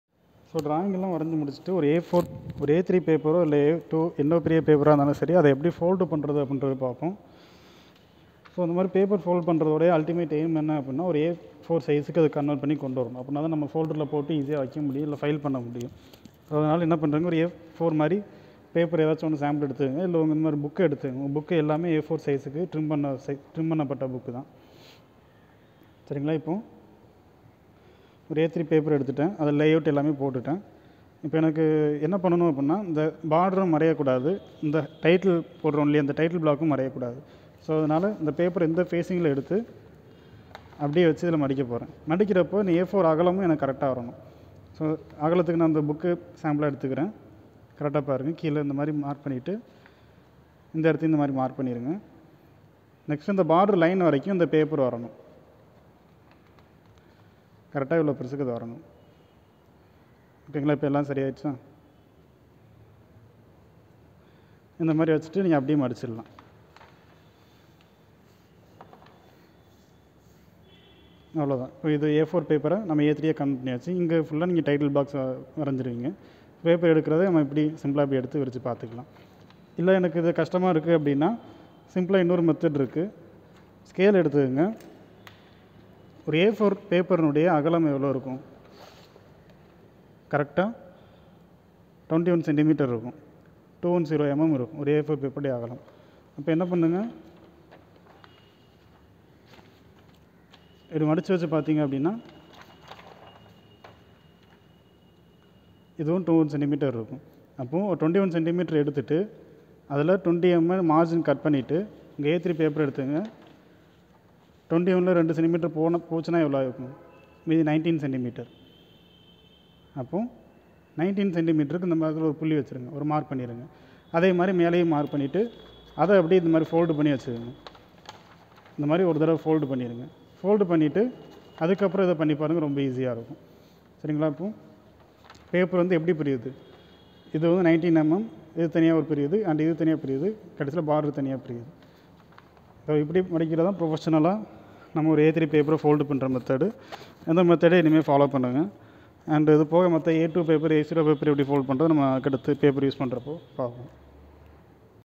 HOW TO FOLD A3 PAPER TO A4 SIZE | A3 பேப்பரை A4 அளவிற்கு மடிப்பது எப்படி | ENGINEERING DRAWING TAMIL
Folding A3 sheet into A4 size for Punching and Filing, A3 size பேப்பரை A4 அளவிற்கு மடிப்பது எப்படி | ENGINEERING DRAWING | DRAUGHTSMAN TRADE | ITI | DIPLOMA PRACTICAL | TAMIL